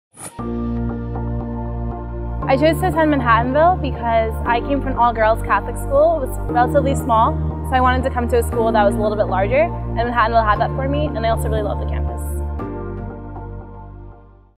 Why Manhattanville -- Chloe Gauthier
Chloe Gauthier '16 shares why she chose Manhattanville.